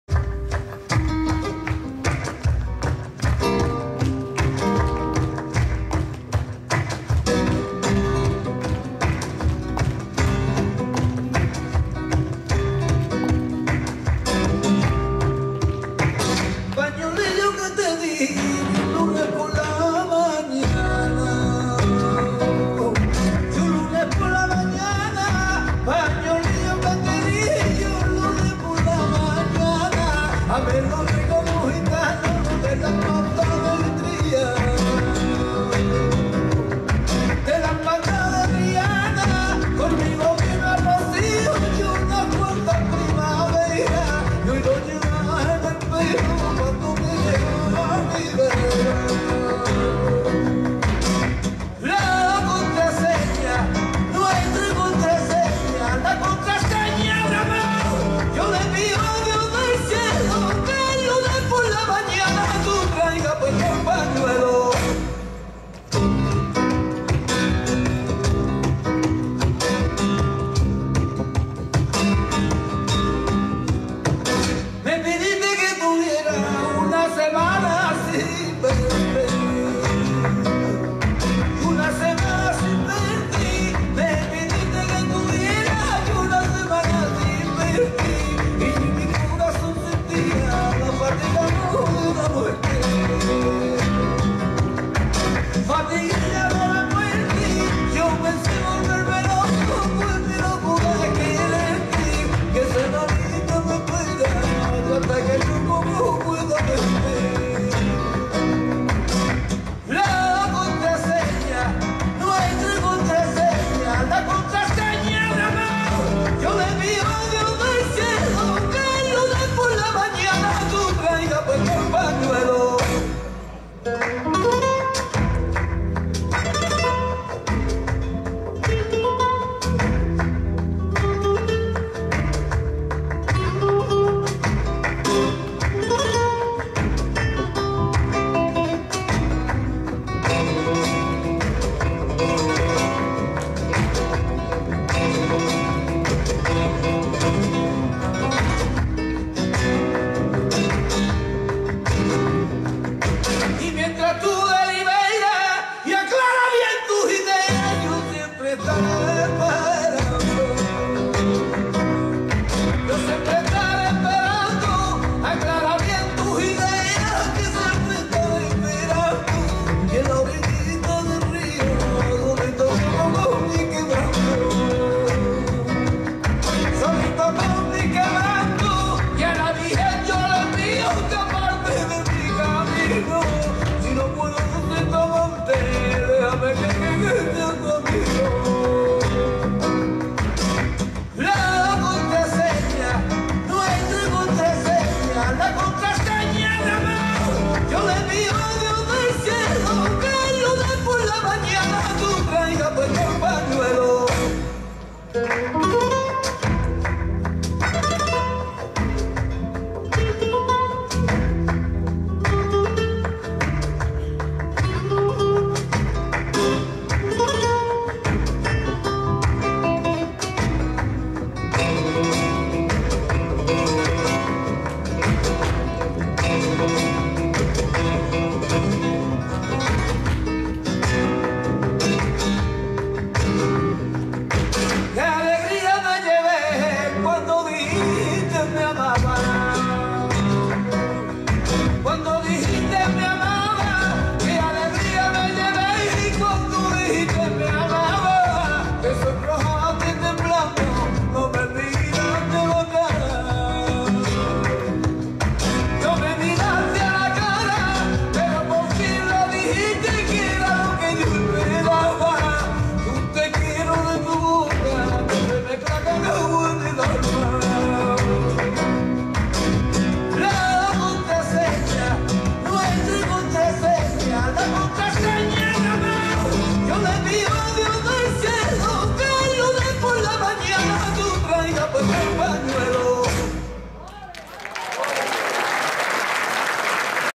Alegria de Triana. La Ciotat 2013
La troupe Alegria de Triana en spectacle, à La Ciotat, salle Paul Eluard, le 15 février 2013.